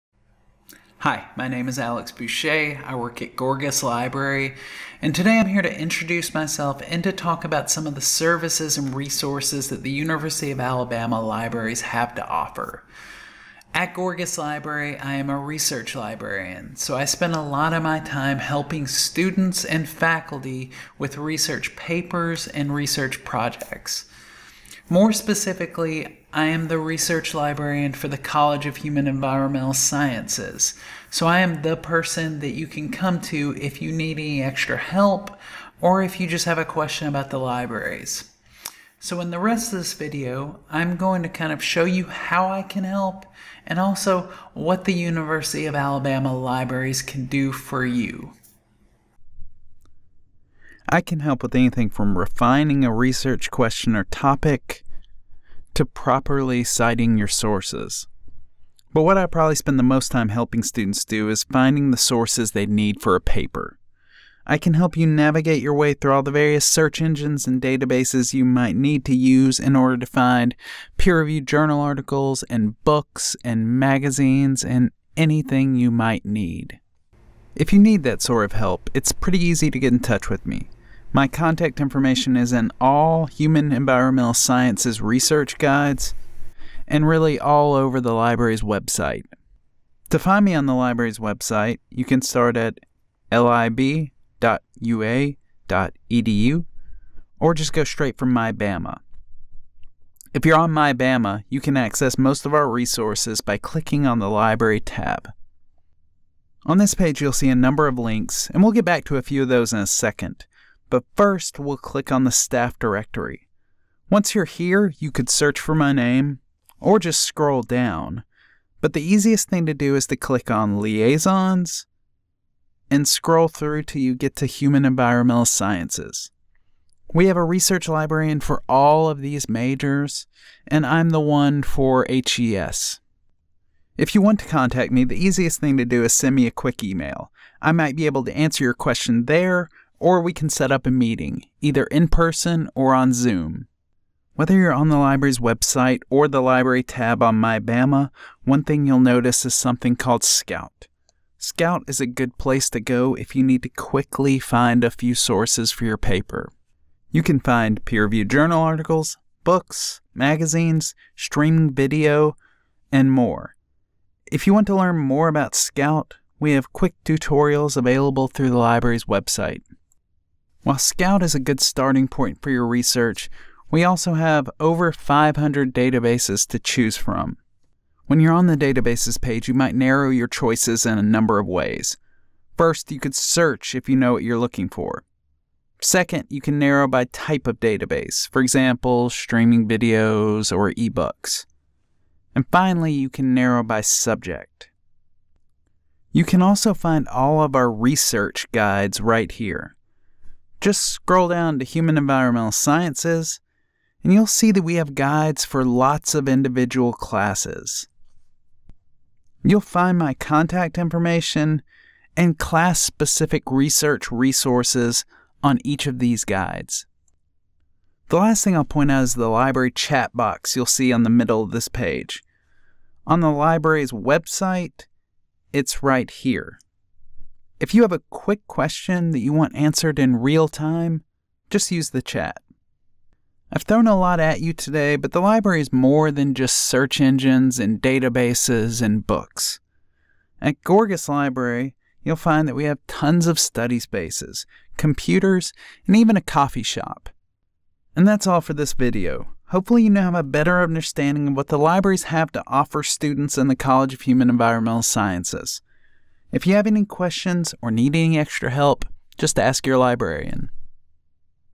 A Guide to the Libraries for CHES Students
In this video, you'll be introduced to some of the services & resources that The University of Alabama Libraries have to offer for students in the College of Human and Environmental Sciences.

Transcript:

Hi, my name is Alex, I work at Gorgas Library, and today I'm here to introduce myself and talk about some of the services & resources that The University of Alabama Libraries have to offer.
At Gorgas Library, I am a research librarian, so I spend a lot of my time helping students & faculty with research projects & research papers.
More specifically I’m the research librarian for the entire College of Human Environmental Sciences, so I’m the person you can come to if you need any help with a research project, or if you just have a question about the libraries.
So in the rest of this video, I'm going to show you how I can help, and what The University of Alabama Libraries can do for you.
I can help with anything from refining a research question or topic…..to properly citing your sources. But what I probably spend the most time helping students do is find the sources they need for a paper. I can help you navigate your way through all the various search engines and databases you might need to use in order to find peer-reviewed journal articles and books and magazines and anything you might need.
If you need that sort of help, it’s pretty easy to get in touch with me – my contact information is in all Human Environmental Sciences Research Guides…and all over the libraries website.
To find me on the libraries website, you can start at lib.ua.edu, or go straight from mybama. If you’re on mybama, you can access most of our resources by clicking on the library tab.
On this page, you’ll see a number of links, and we’ll get back to a few of those in a second. But first, we’ll click on the staff directory. Once you’re here, you could search for my name, or just scroll down, but the easiest thing to do is to click on liaisons and scroll through till you get to Human Environmental Sciences. We have a research librarian for all of these majors, and I’m the one for HES.
I might be able to answer your question there, or we can set up a meeting – either in-person or on zoom.
Now that you know how to get in touch with me, let’s run through a few things on the libraries website and this research guide.
Whether you’re on the libraries website or on the library tab on mybama, one thing you’ll notice is something called Scout. Scout is a good place to go if you need to quickly find a few sources for a paper.
You can find peer-reviewed journal articles, books, magazines, streaming video – among other things. If you want to learn more about Scout, we have quick tutorials available through the libraries website.
While Scout is a good starting point for your research, we also have over 500 databases to choose from.
When you’re on the databases page, you might narrow your choices in a number of ways. One, you could search if you know what you’re looking for. Two, you can narrow by type of database – for example, streaming video, or eBooks. And finally, you can narrow by subject.
You can also find all of our research guides right here. Just scroll down to Human Environmental Sciences and you’ll see that we have guides for lots of individual classes.
The last thing I’ll point out is the library chat box you’ll see on the middle of the page. On the library’s website it’s right here. If you have a quick question that you want answered in real time, just use the chat.
I’ve thrown a lot at you today, but the library is more than just search engines & databases. At Gorgas Library, you’ll find that we have tons of study spaces, computers, and even a coffee shop.
And that’s all we’ve got for this video. Hopefully you now have a better understanding of what the libraries have to offer students in The College of Human Environmental Sciences. If you have any questions or need any extra help, just ask your librarian.